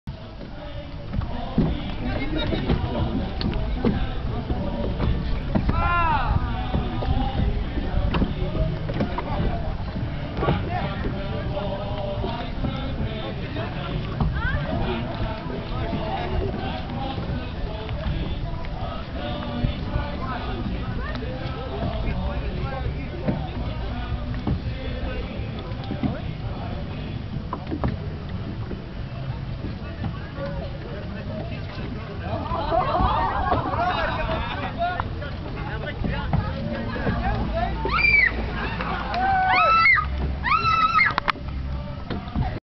Land far away but I can hear the music in Guernsey
Drifting in the waters in front of the island of Guernsey.